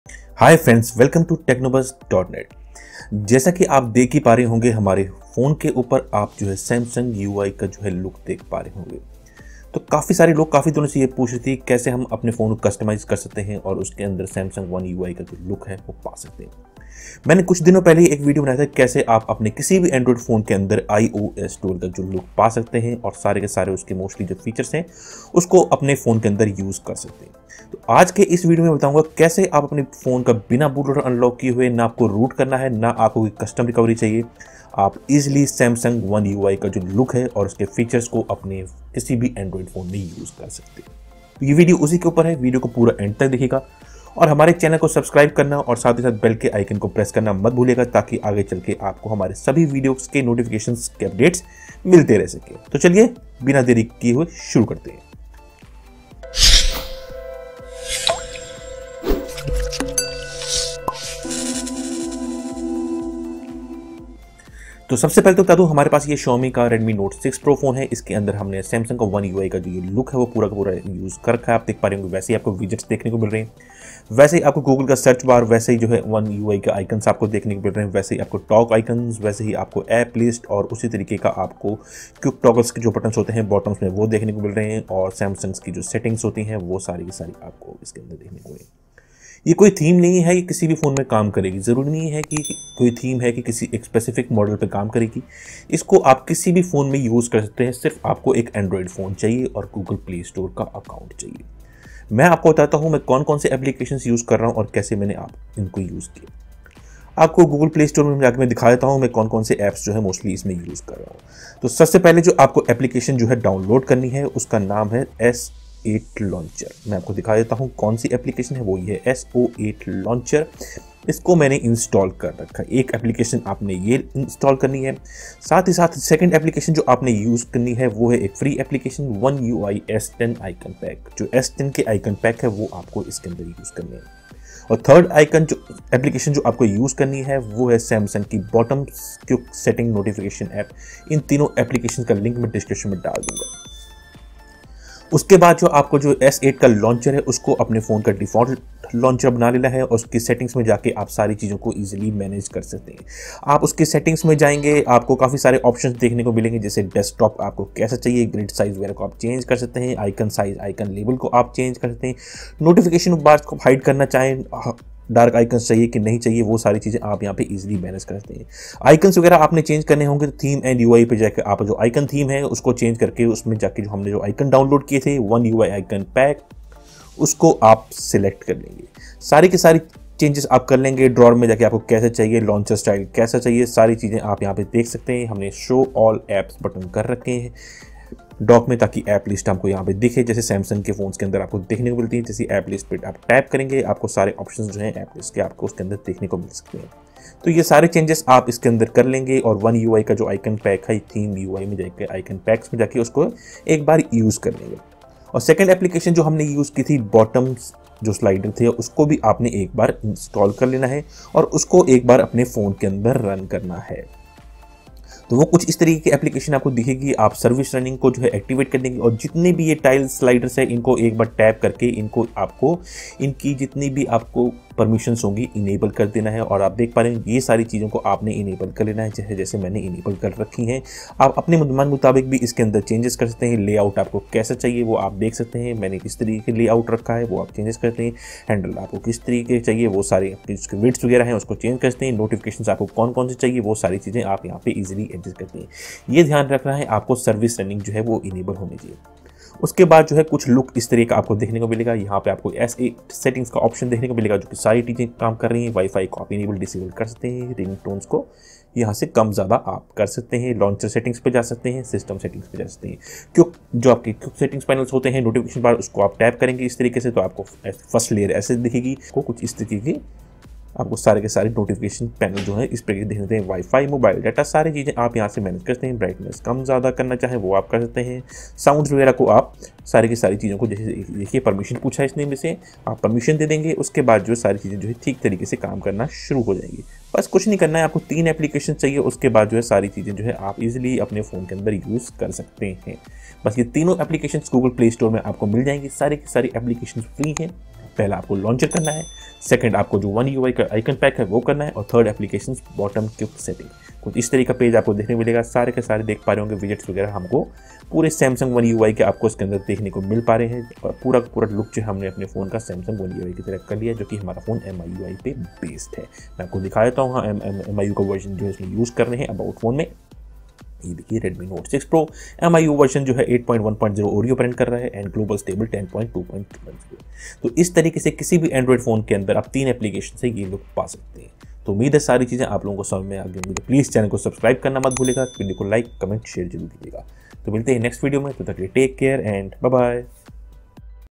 How to Get Samsung's One Ui Look on Any ANDROID PHONE
How to Get Samsung's One Ui Look on Any ANDROID PHONE. No need to Root on install custom recovery to get this look. This will work on any androdi mobile.